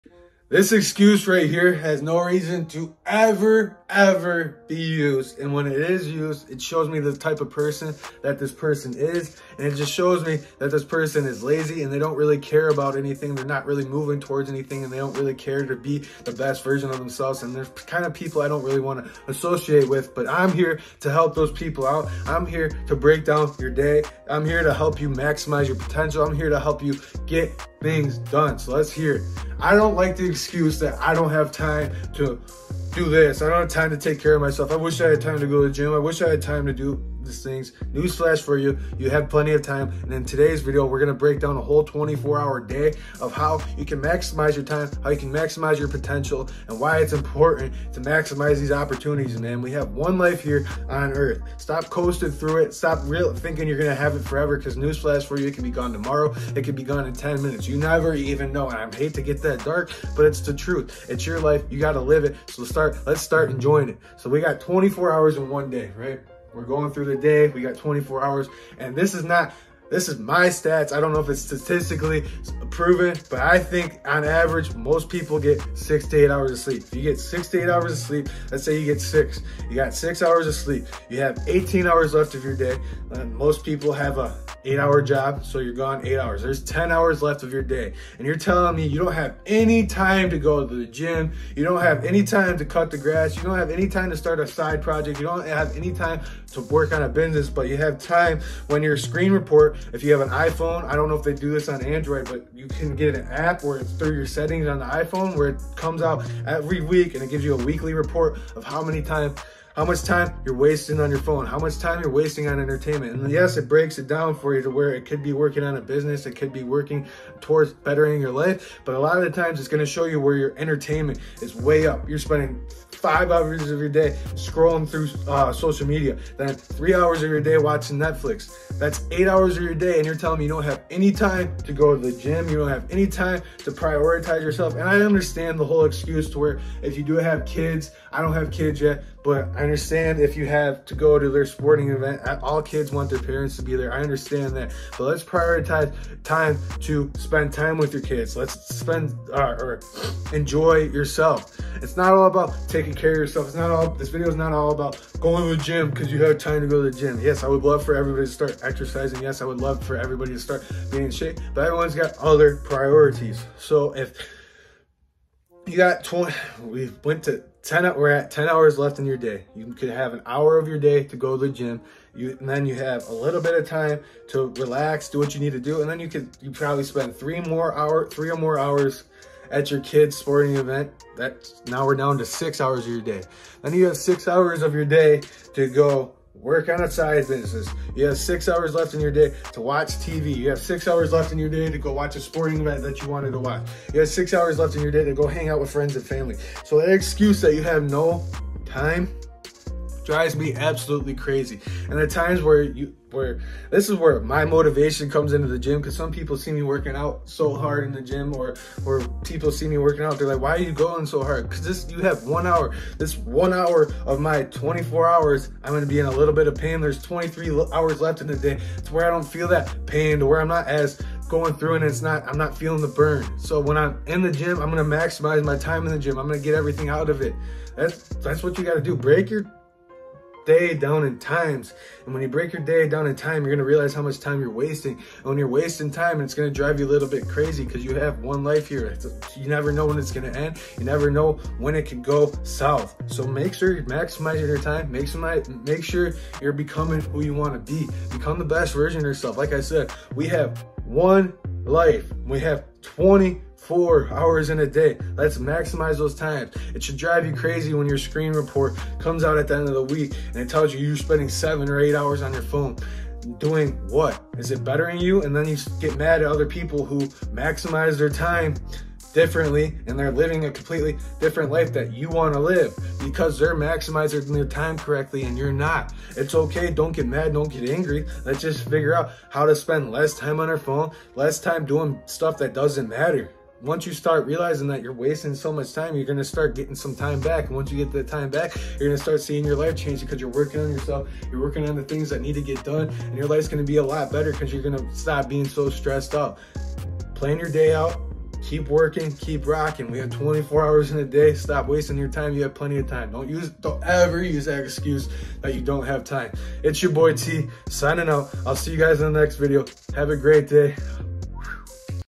🚨 MOTIVATION: YOU HAVE PLENTY OF TIME🚨
• In todays video I breakdown a whole 24 hour day to explain how much time is being wasted and why it should drive you crazy how much time is being wasted